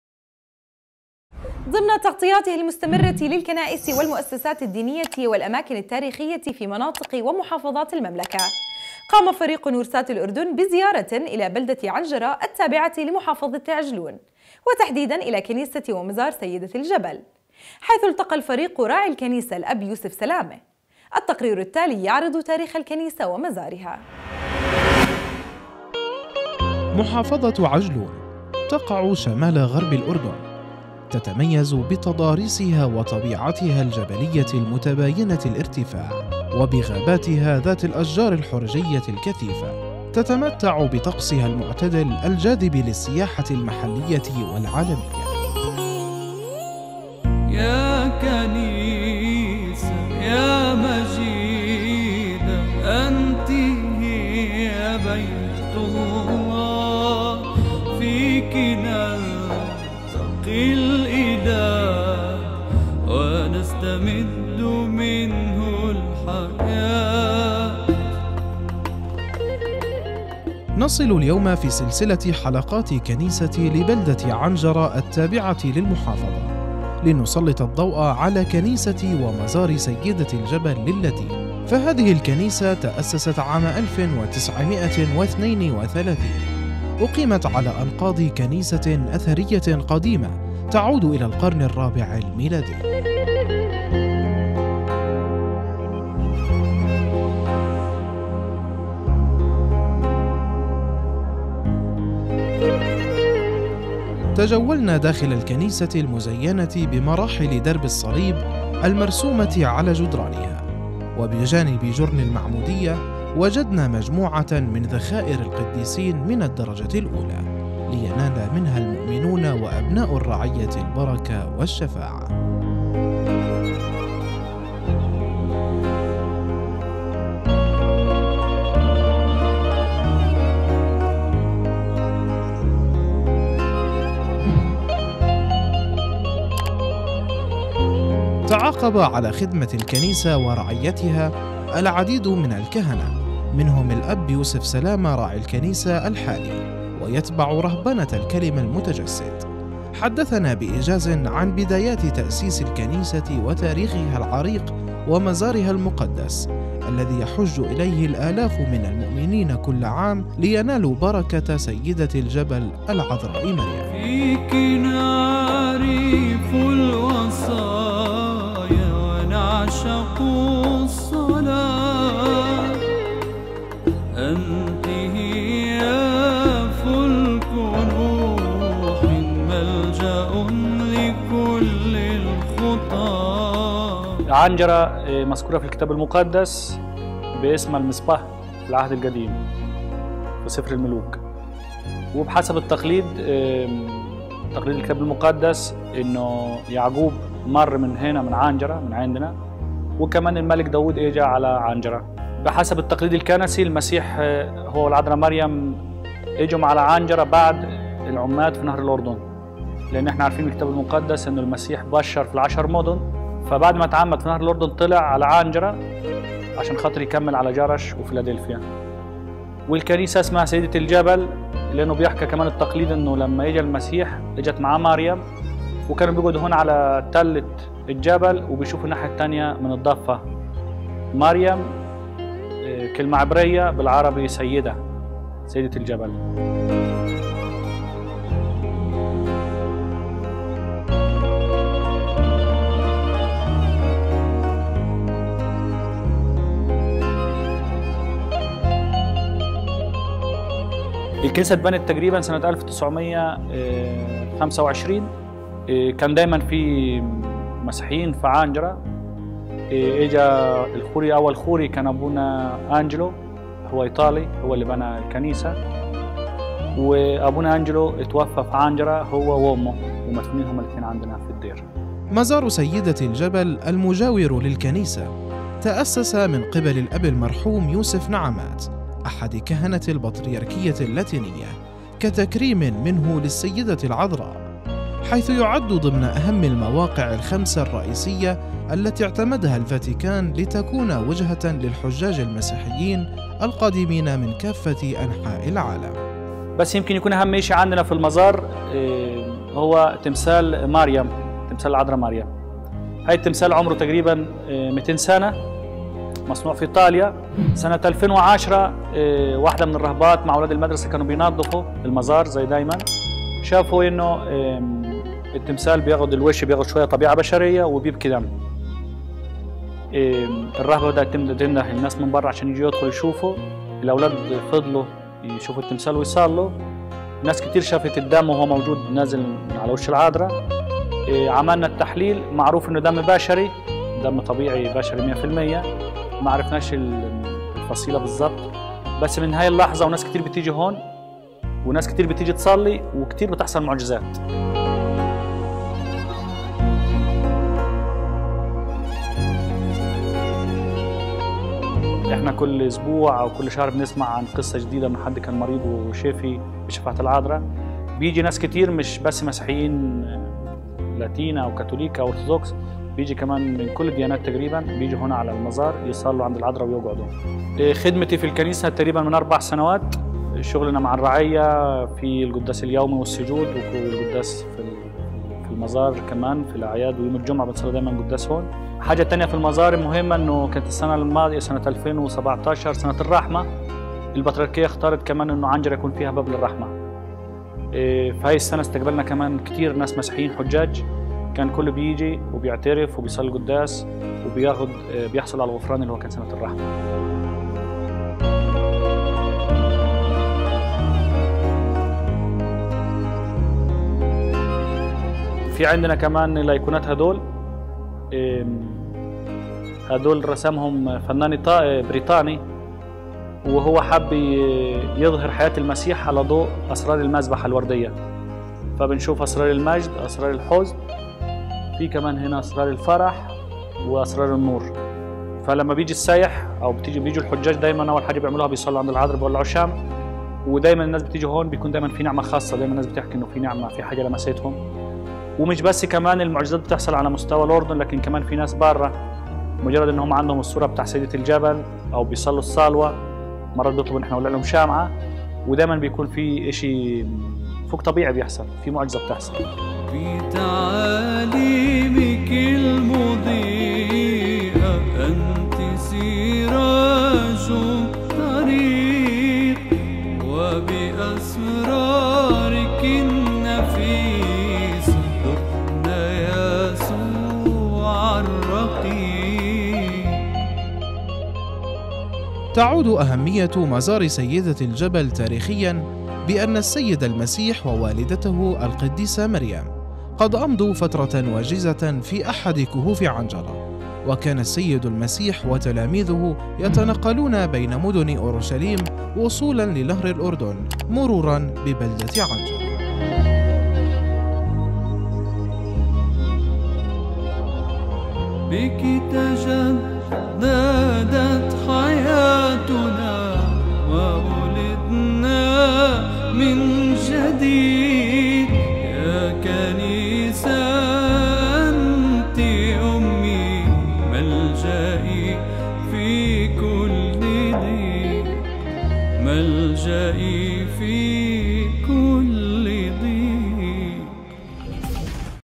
نورسات الأردن : فريقُ نورسات الأردن بزيارةٍ الى بلدةِ عنجره - عجلون / كنيسةِ ومزارِ سيدةِ الجبل
ضمن تغطياتهِ المستمرةِ للكنائسِ والمؤسساتِ الدينيةِ والأماكنِ التاريخيةِ في مناطقِ ومحافظاتِ المملكه / قام فريقُ نورسات الأردن بزيارةٍ الى بلدةِ عنجره التابعةِ لمحافظةِ عجلون / وتحديداً الى كنيسةِ ومزارِ سيدةِ الجبل / حيث التقى الفريقُ راعي الكنيسه الاب يوسف سلامه / التقريرُ التالي يعرضُ تاريخَ الكنيسةِ ومزارِها